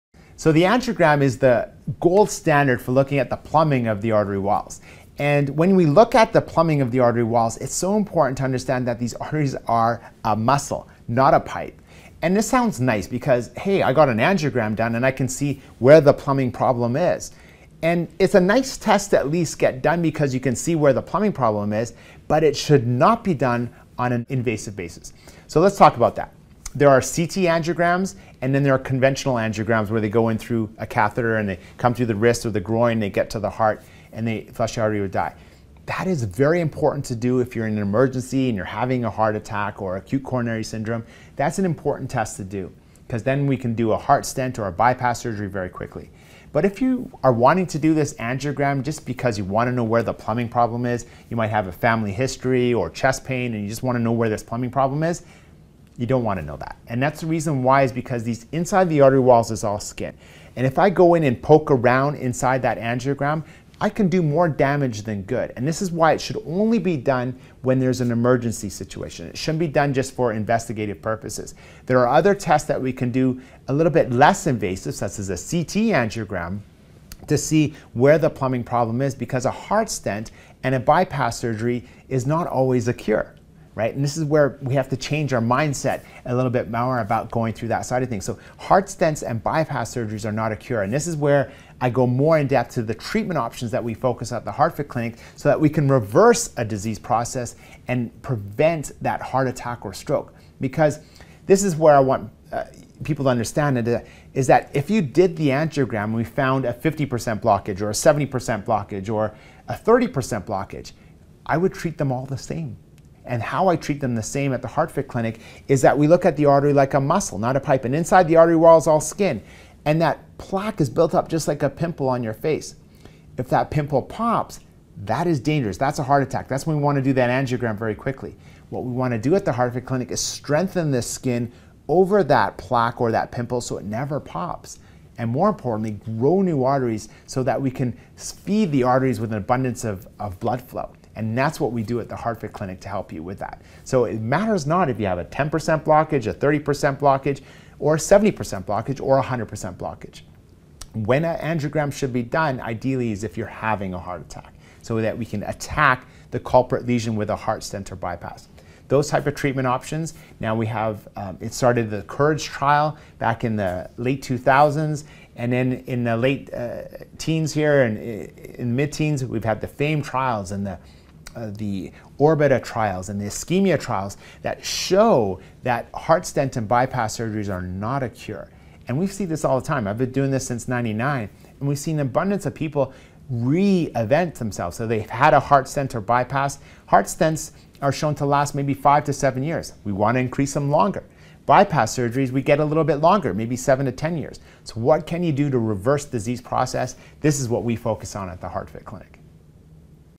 When you should and should not go for an ANGIOGRAM
Reverse heart disease and heart disease treatment.
Cardiac rehabilitation program after a heart attack, heart stent or heart bypass surgery.
Heart stents and bypass surgery are not a cure. Therefore we should be choosing wisely exposing patients to an angiogram or CT angiogram.